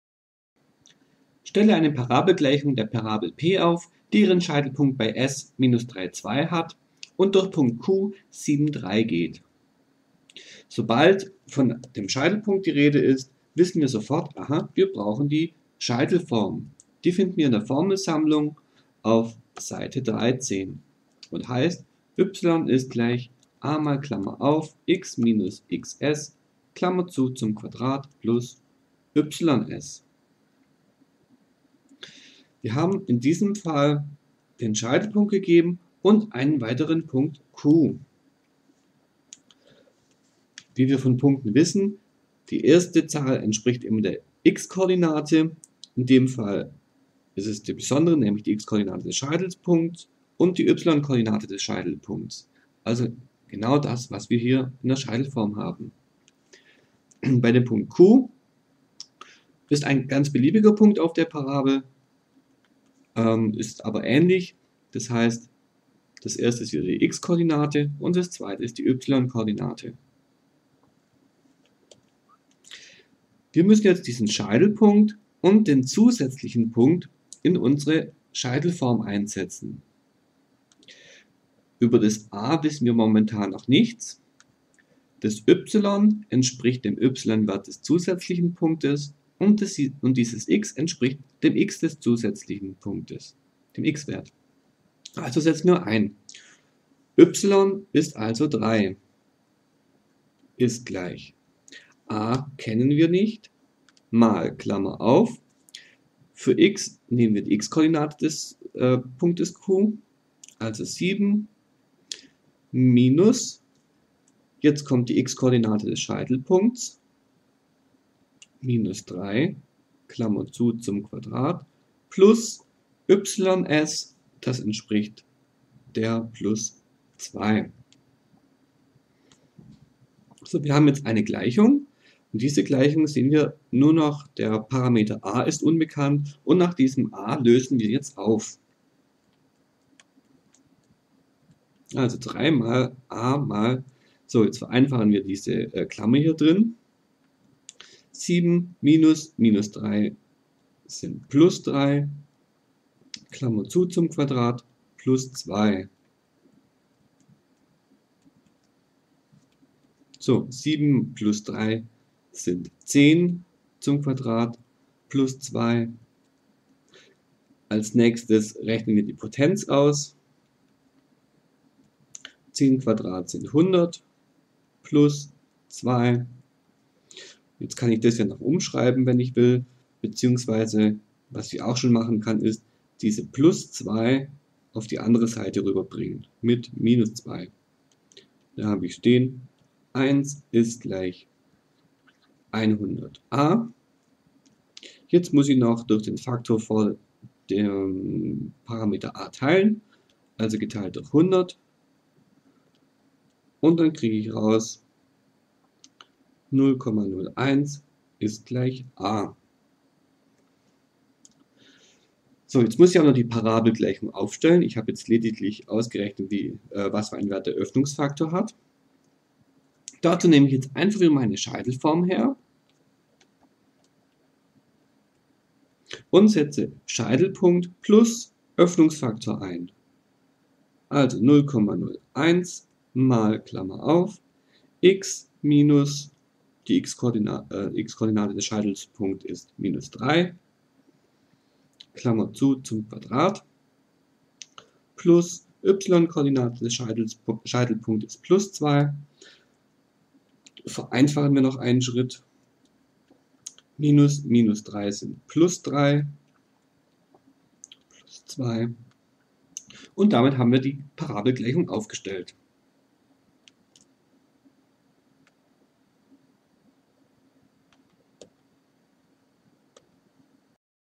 Parabel - Gleichung aufstellen mit Scheitelpunkt und einem weiteren Punkt (9I.3 | 10II.1)
Wie stelle ich eine Parabelgleichung auf, wenn:
- der Scheitelpunkt gegeben ist
- ein weiterer beliebiger Punkt der Parabel gegeben ist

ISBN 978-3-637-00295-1